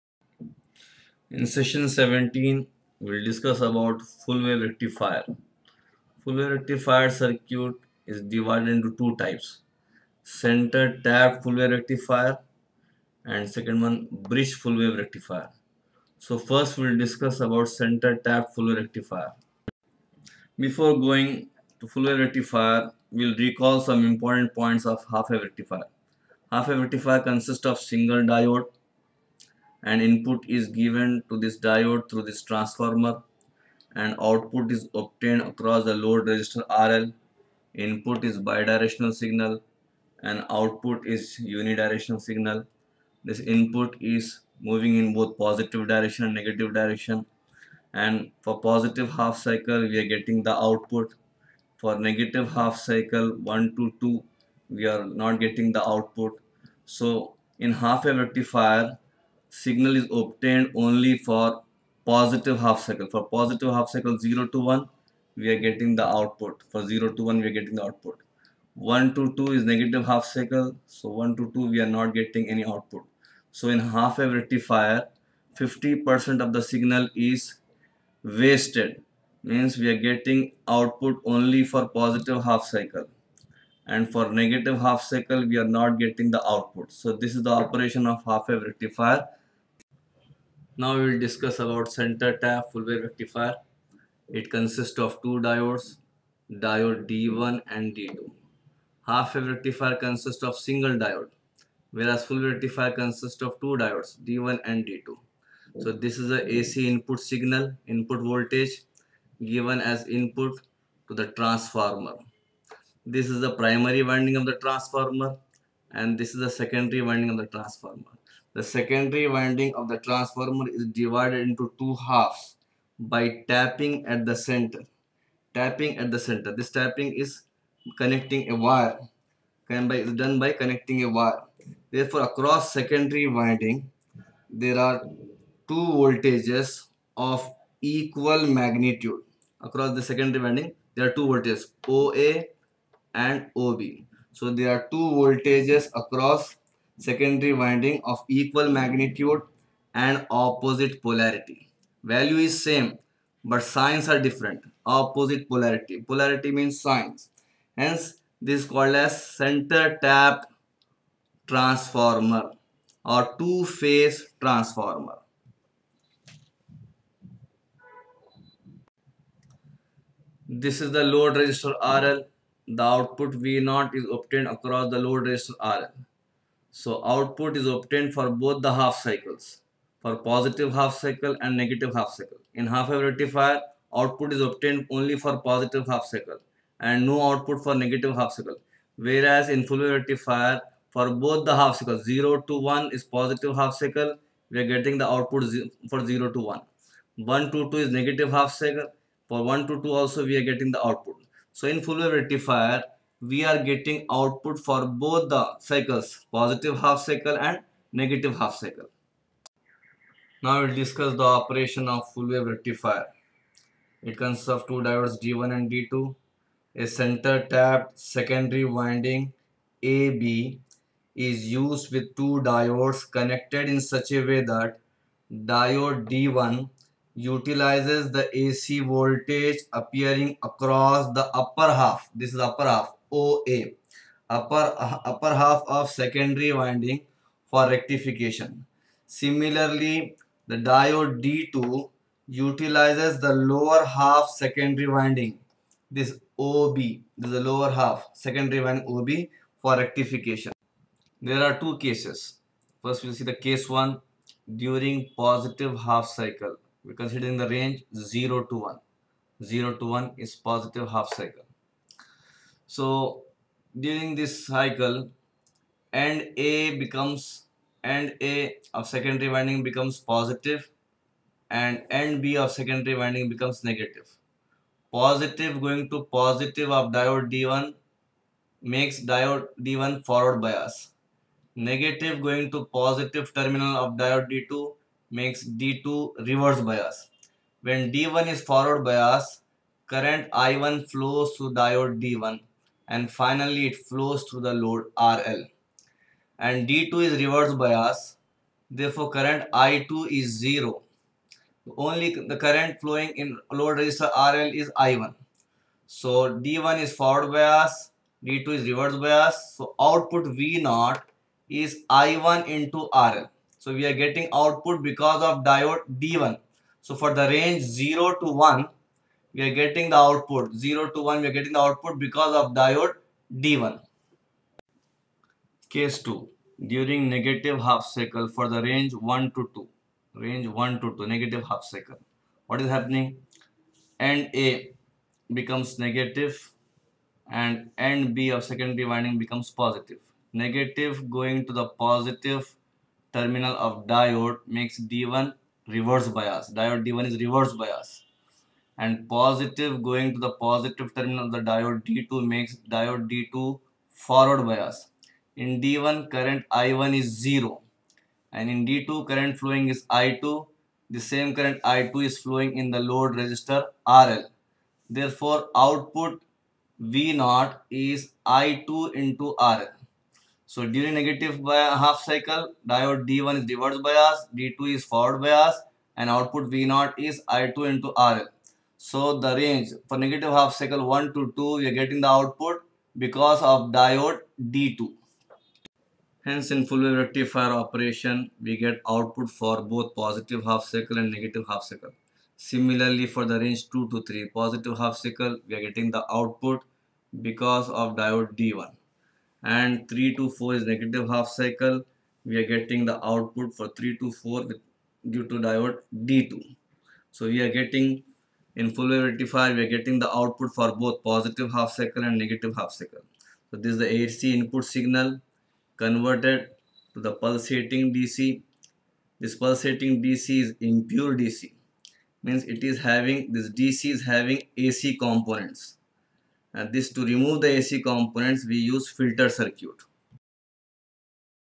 BASIC ELECTRONICS (SESSION-17) CENTRE-TAPPED FULL WAVE RECTIFIER FOR(INTER, BE/BTECH) BY ASIF ANSARI
FULL WAVE  RECTIFIER (FWR):-

IN FWR  ,THE OUTPUT IS OBTAINED FOR  BOTH THE HALF  CYCLES (POSITIVE HALF CYCLE AND NEGATIVE HALF CYCLE) UNLIKE  HWR.
IN HWR,  THE OUTPUT IS OBTAINED ONLY FOR POSITIVE HALF  CYCLE. AND NO OUTPUT FOR  NEGATIVE  HALF CYCLE.

FULL WAVE RECTIFIER ARE OF TWO TYPES
1) CENTRE-TAPPED  FULL WAVE RECTIFIER (FWR).
2) BRIDGE FULL WAVE RECTIFIER (FWR)

CENTRE -TAPPED FULL WAVE RECTIFIER (FWR):-

IT CONSISTS OF TWO DIODES D1 AND D2.

INPUT VOLTAGE IS APPLIED TO FWR THROUGH CENTRE TAPPED  TRANSFORMER.

THE SECONDARY WINDING OF TRANSFORMER IS DIVIDED INTO TWO EQUAL HALVES BY CONNECTING A WIRE AT THE CENTRE.
THIS IS CALLED AS  CENTRE -TAPPING .

DUE TO THIS ,TWO VOLTAGES OF EQUAL MAGNITUDE  AND OPPOSITE  POLARITY (SIGN) ARE OBTAINED AT SECONDARY WINDING OF THE TRANSFORMER .

THE OUTPUT IS OBTAINED ACROSS THE LOAD RESISTOR (RL).